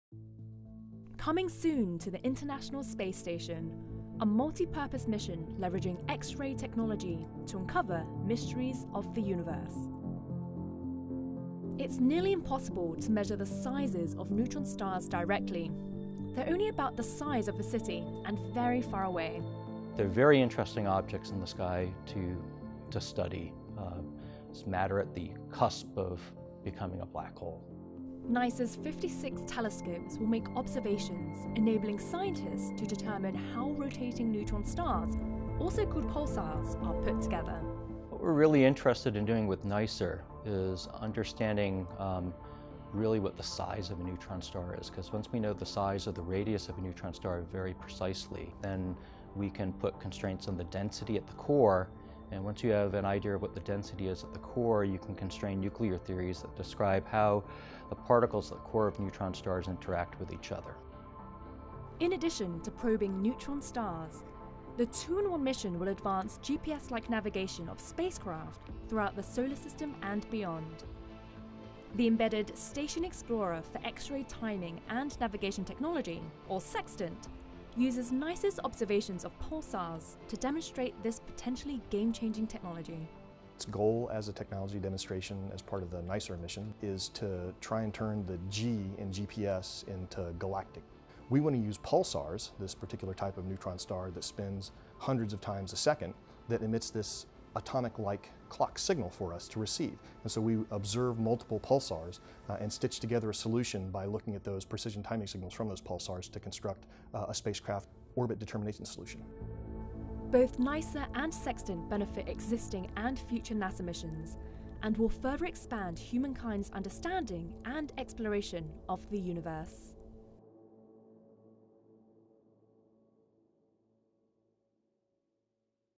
NICER Mission Overview
The Neutron Star Interior Composition Explorer (NICER) payload, destined for the exterior of the space station, will study the physics of neutron stars, providing new insight into their nature and behavior. These stars are called “pulsars” because of the unique way they emit light – in a beam similar to a lighthouse beacon. As the star spins, the light sweeps past us, making it appear as if the star is pulsing. Neutron stars emit X-ray radiation, enabling the NICER technology to observe and record information about their structure, dynamics and energetics. The payload also includes a technology demonstration called the Station Explorer for X-ray Timing and Navigation Technology (SEXTANT) which will help researchers to develop a pulsar-based space navigation system. Pulsar navigation could work similarly to GPS on Earth, providing precise position and time for spacecraft throughout the solar system.

The 2-in-1 mission is scheduled to launch on June 1, 2017, at 5:55 p.m. EDT aboard SpaceX's eleventh contracted cargo resupply mission with NASA to the International Space Station. If successfully launched on June 1, the payload will arrive at the space station in the Dragon spacecraft, along with other cargo, on June 4, 2017.

Credit: NASA’s Goddard Space Flight Center/Clare Skelly

This video is public domain and along with other supporting visualizations can be downloaded from the Scientific Visualization Studio